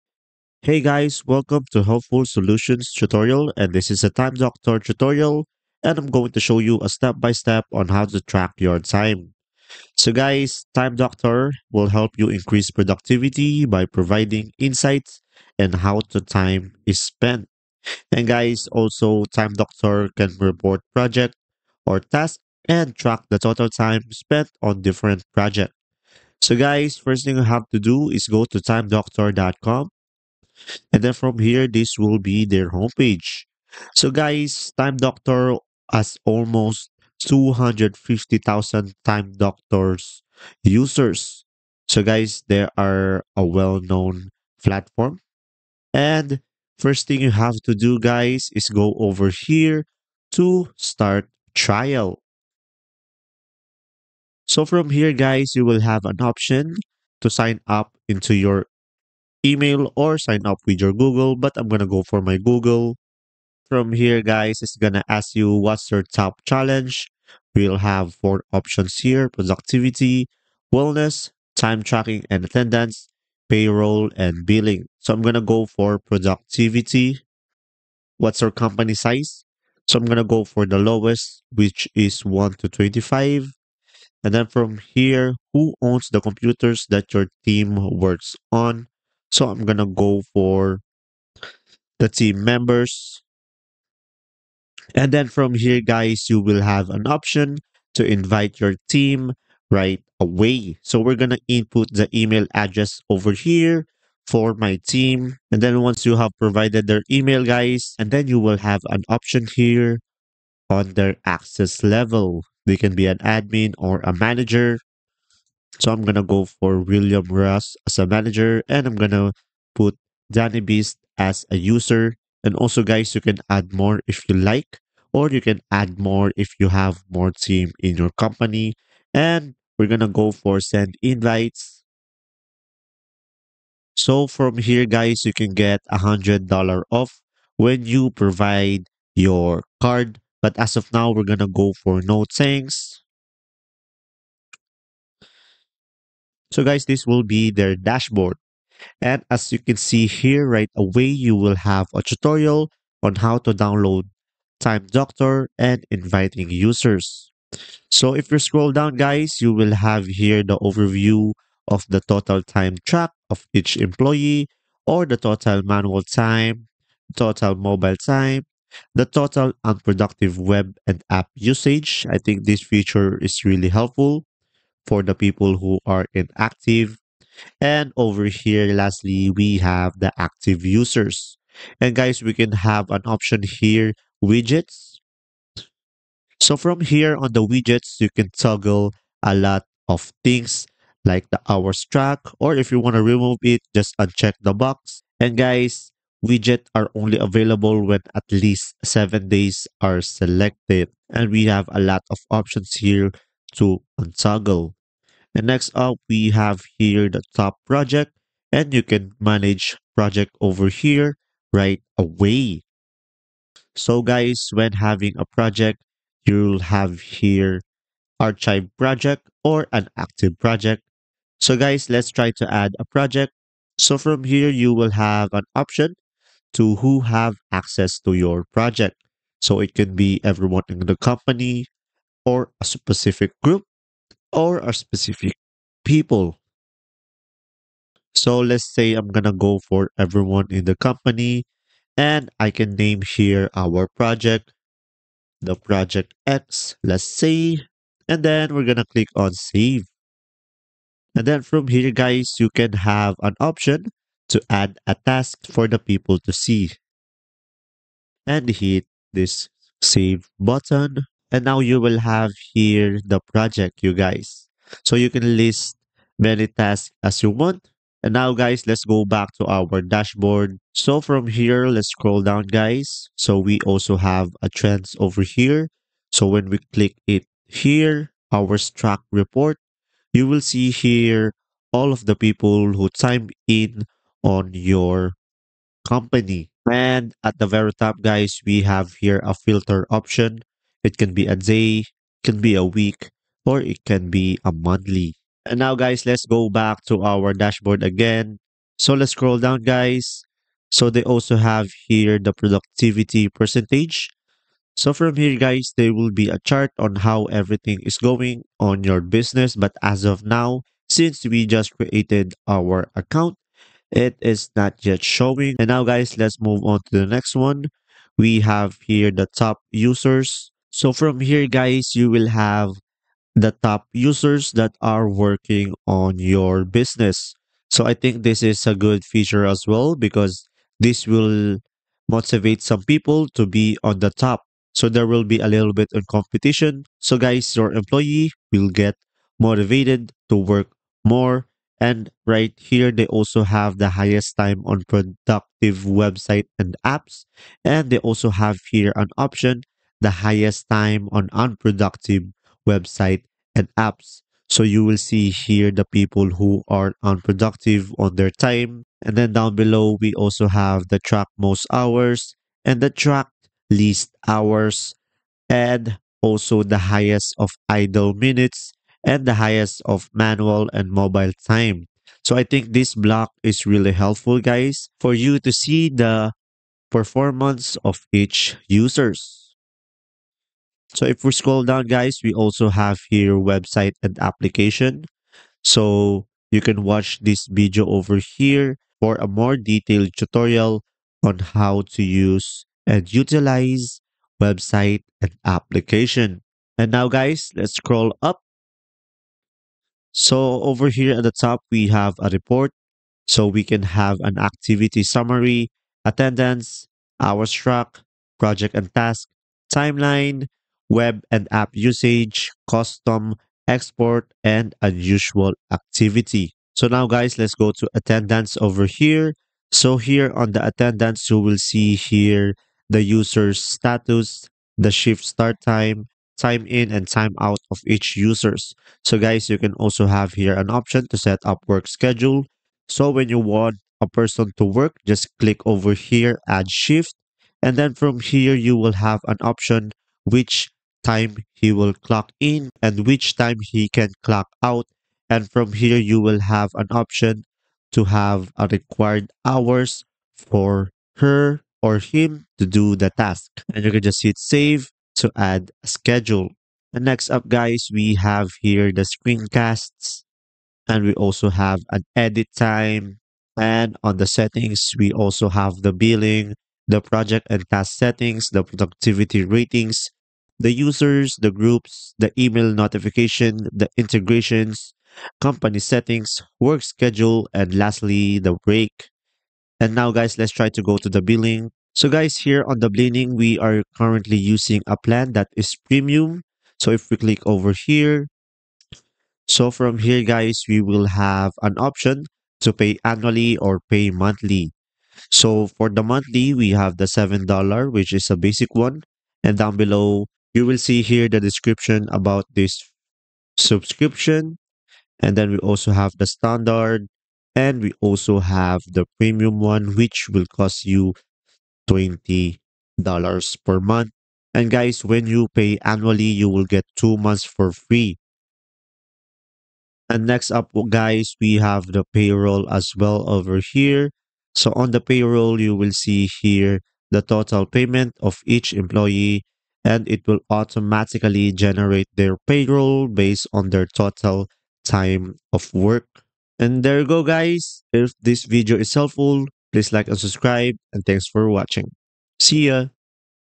Time Doctor Tutorial 2024 For Beginners (How To Use Time Doctor To Track Your Time)
Time Doctor Tutorial 2024 For Beginners (How To Use Time Doctor To Track Your Time). In this video tutorial I will show how to use Time Doctor to track your time.